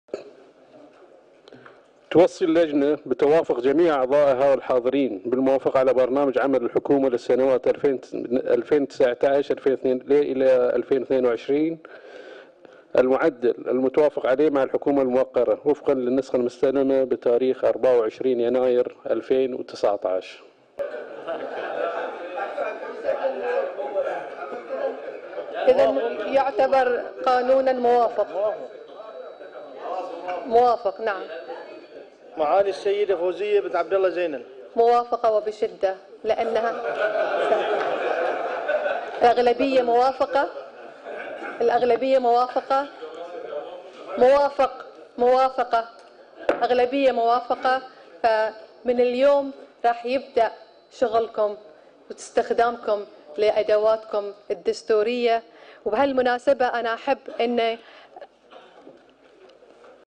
البحرين | بالفيديو ... "النواب" يقر برنامج عمل الحكومة .. والرئيسة: أوافق بشدة والعمل يبدأ اليوم
البحرين |"النواب" يقر برنامج عمل الحكومة .. والرئيسة: أوافق بشدة والعمل يبدأ اليوم .
 الثلثاء(٢٩يناير/ كانون الثاني ٢٠١٩) .
وافق مجلس لتوالي على برنامج عمل الحكومة للأعوام حتى 2022. وقالت رئيسة المجلس. فوزية زينل للنواب عملكم يبدا اليوم للرقابة على عمل عمل الحكومة وتنفيذه ، وقالت في إجابتها على التصويت "أوافق بشدة".